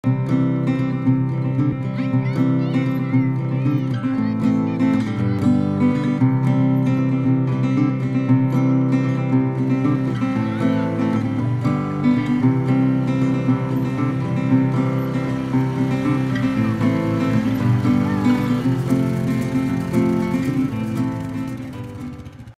Popping a wheely with Grandma
Lol they started rolling back then belly hit the gas. Good thing Zion wasn't standing up at that point of he'd have done a backflip on his ninja turtle head.